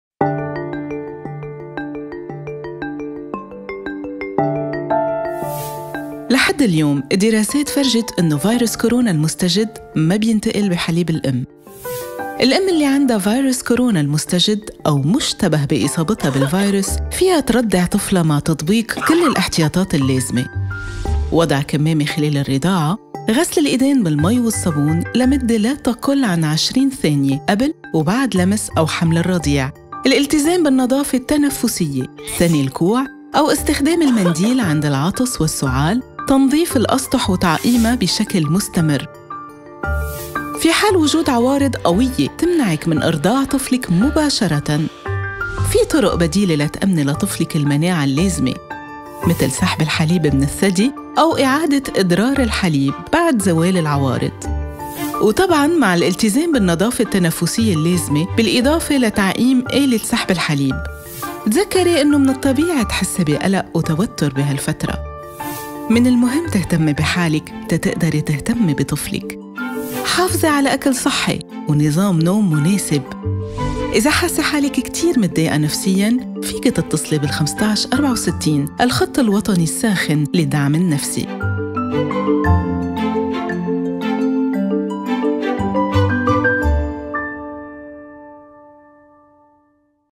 COVID19 Breastfeeding messages
الرضاعة الطبيعية بتحمي المولود من الأمراض لأنها بتقوي جهاز المناعة.
والاتصال الوثيق مع المولود ورضاعته من الثدي بتساعدو على نموّ  أفضل.